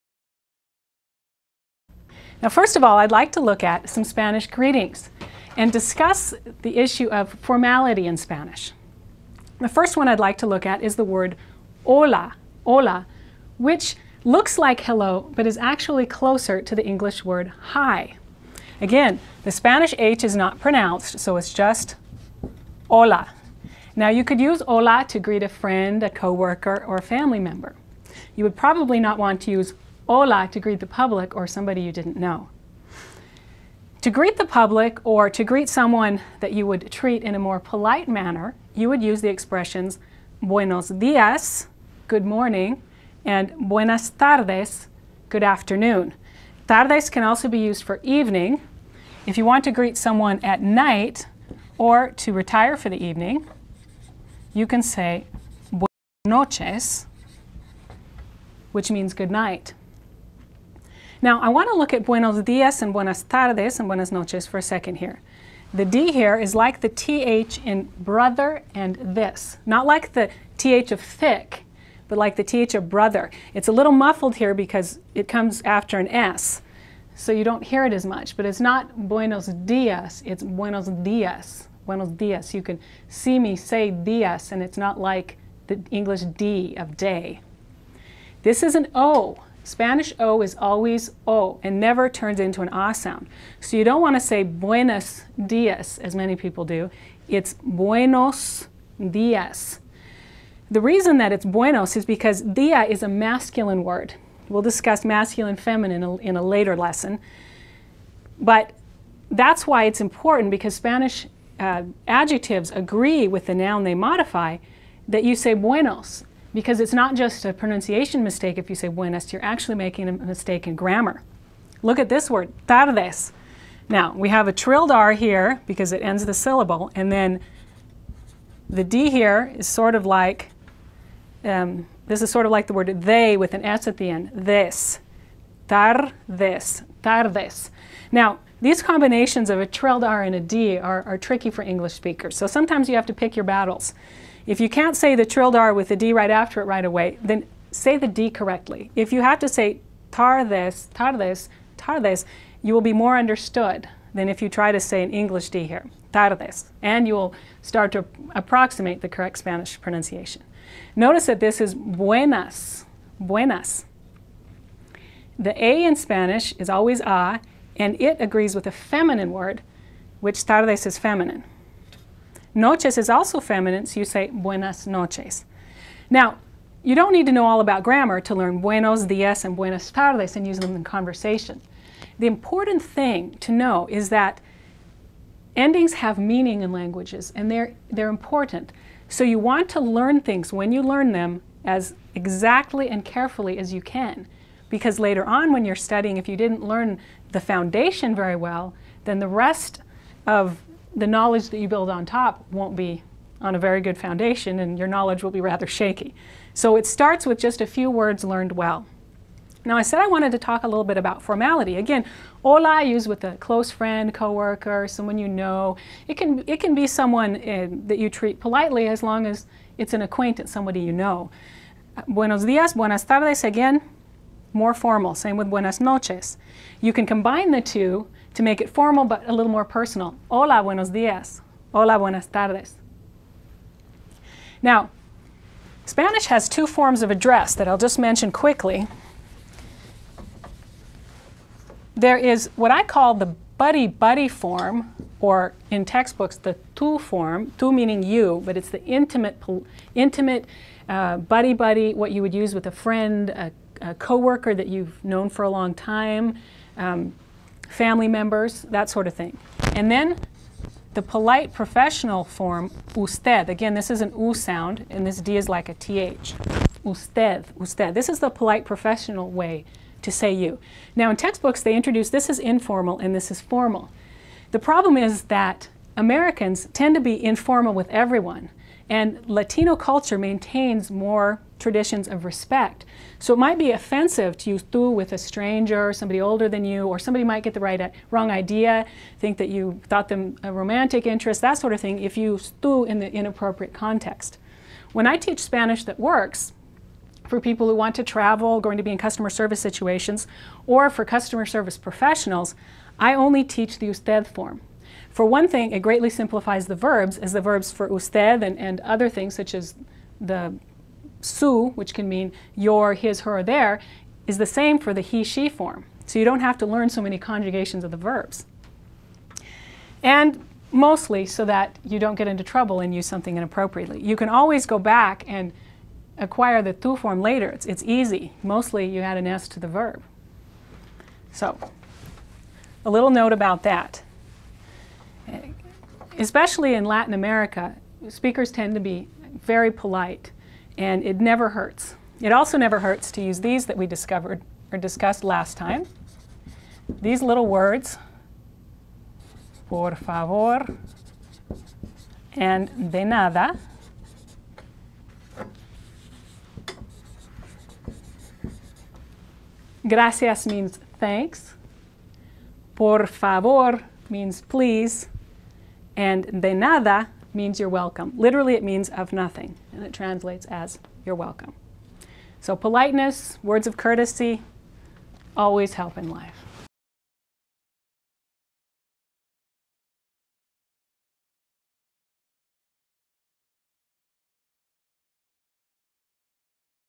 7_Greetings & Courtesy - Spanish that Works Lesson 2 - #3
Expressions for Lessons 2 & 3 of "Spanish that Works for You" by Elizabeth Almann. for free handouts and lessons. More videos on SpanishThatWorksOrg Channel.